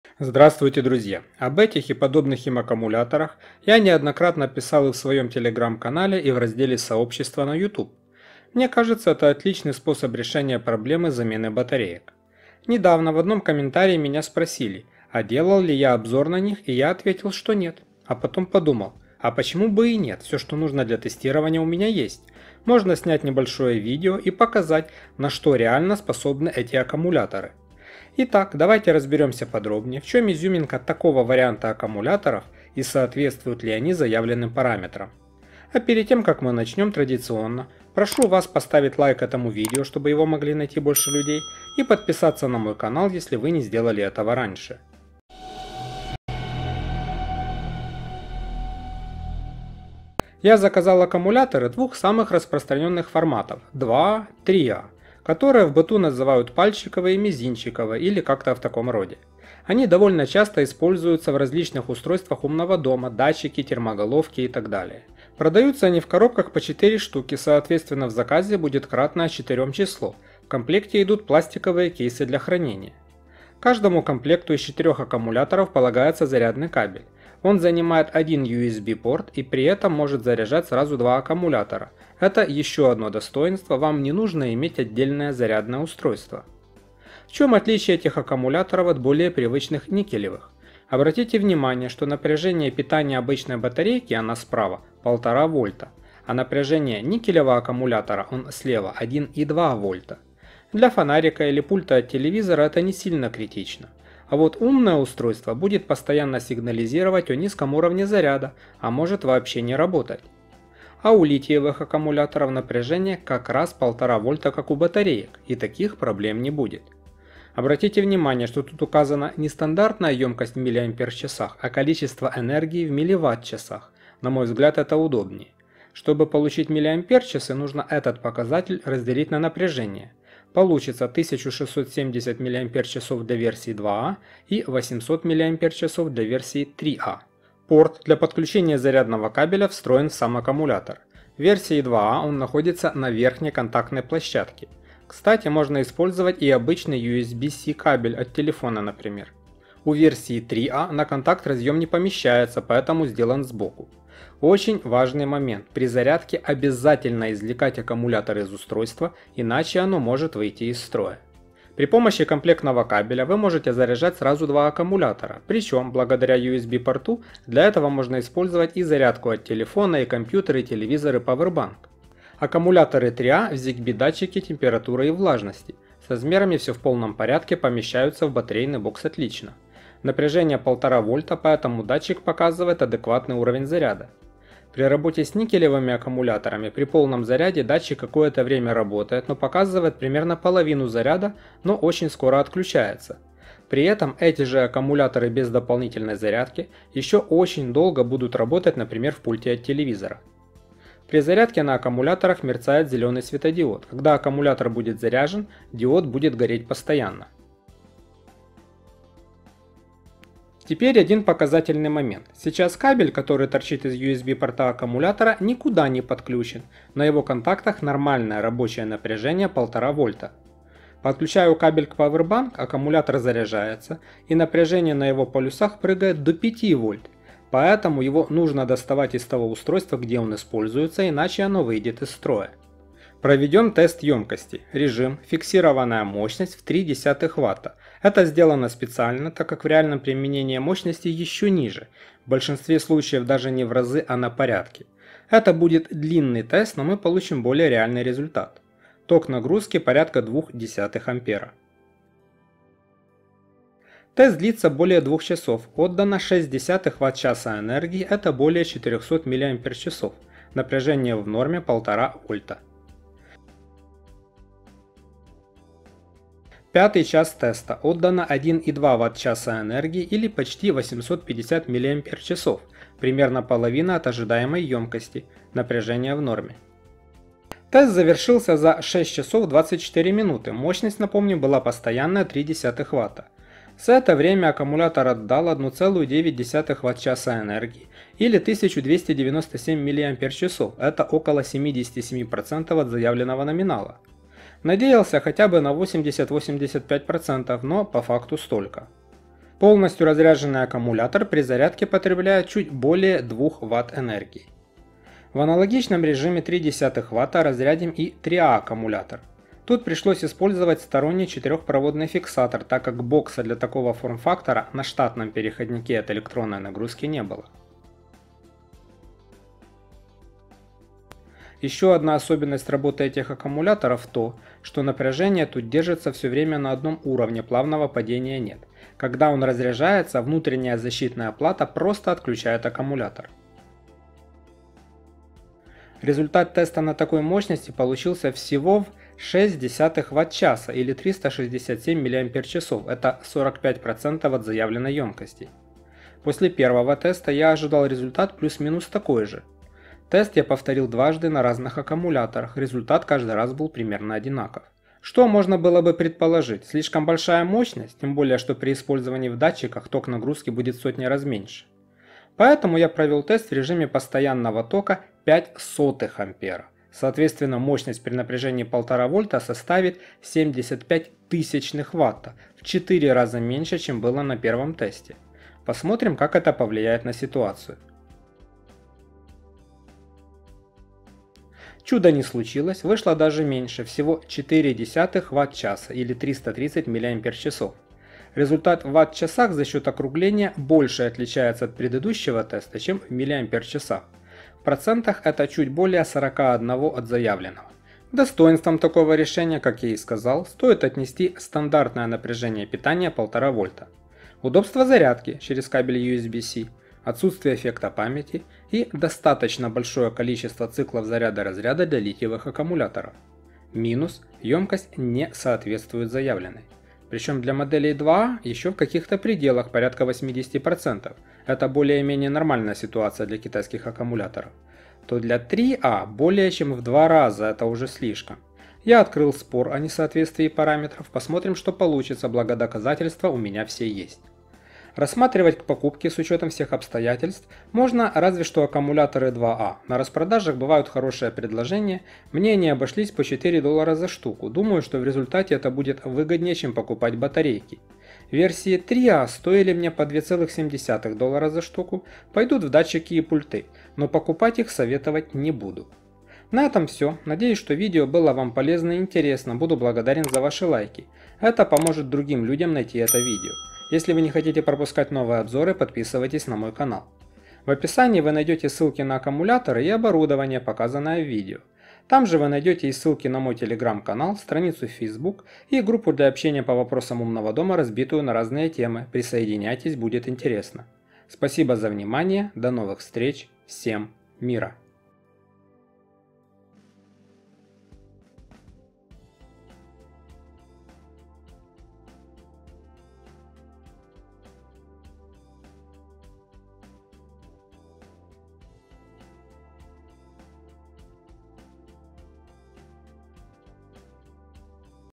Аккумуляторы HAKADI Li Ion - AA и AAA на 1,5 Вольта, с зарядкой по USB C, честный тест
Небольшое видео посвященное литиевым аккумуляторам формата АА и ААА c напряжением питания 1,5 вольта - как у обычных батареек и зарядкой по USB. Отличный вариант для установки в различные гаджеты умного дома - датчики, термоголовки, ИК контроллеры и т.п.

00:00 - Вступление
00:50 - Поставка
01:26 - Параметры и особенности
03:52 - Тест емкости
06:38 - Вывод